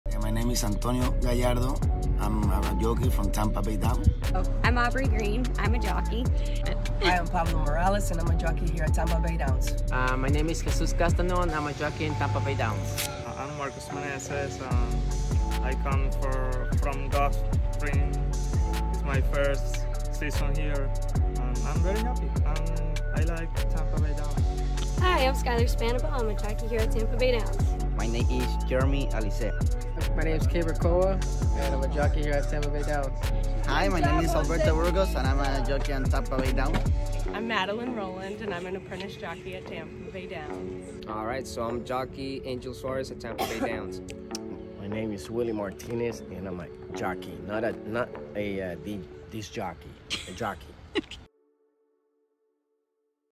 And Now - Say Hello to Tampa Bay Down's Jockey Colony
Meet the jockey riding the horses at Tampa Bay Downs